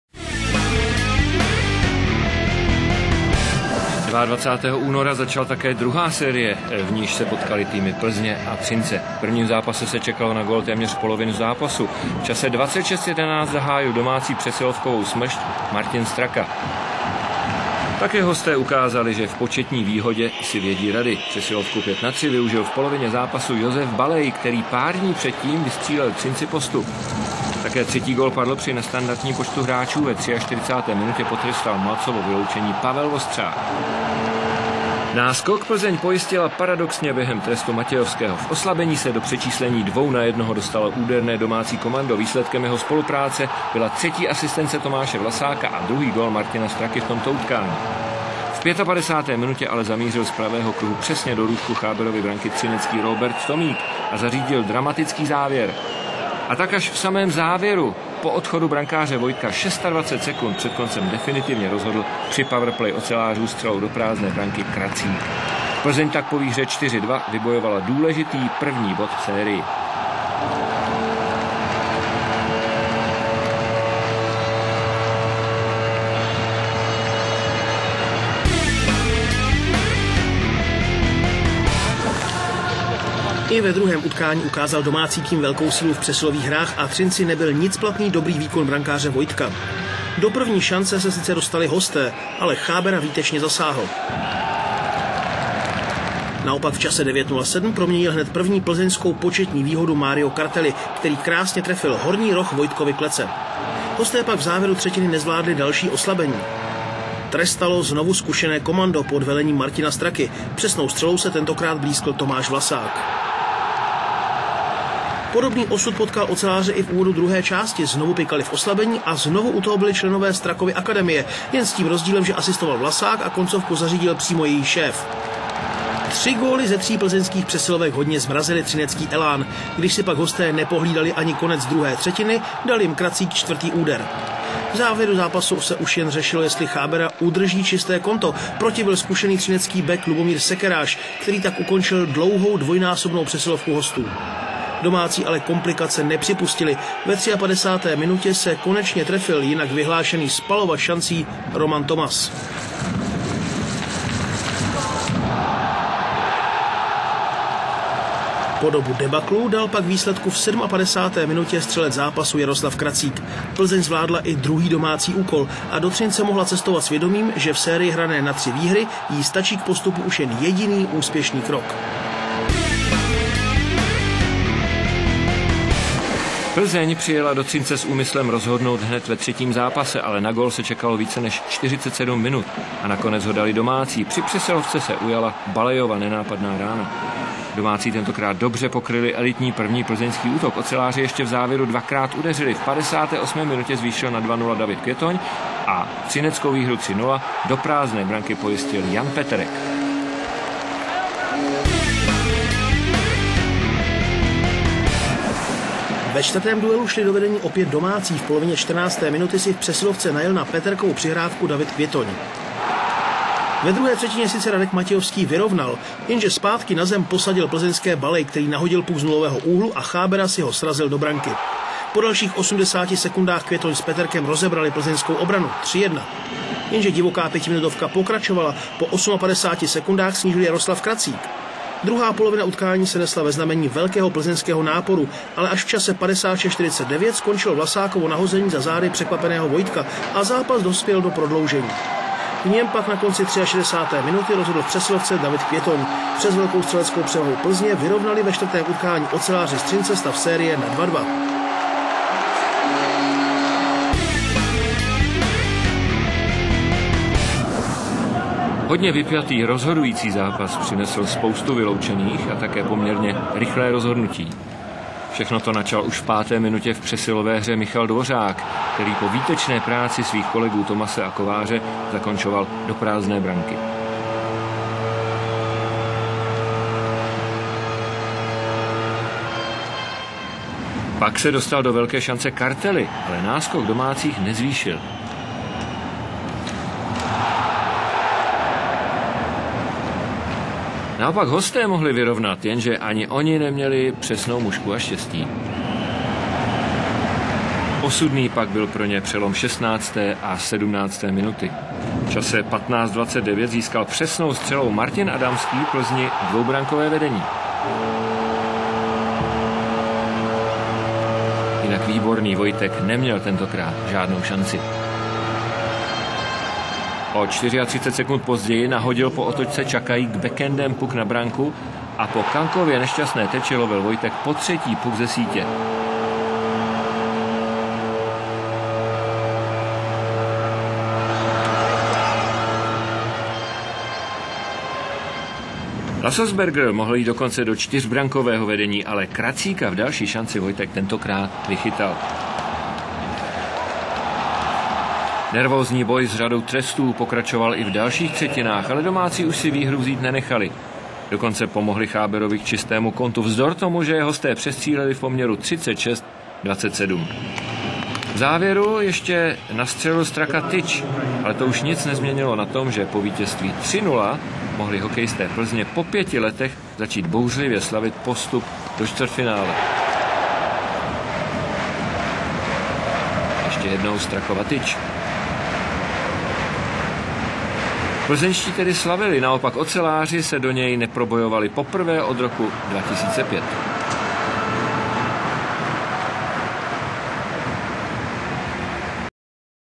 Play off O2 extraligy 2008/2009 - předkolo: HC Lasselsberger Plzeň vs. HC Oceláři Třinec
Vyrobila a vydala GRANT - Produkce s.r.o. ve spolupráci s Českou televizí, 2009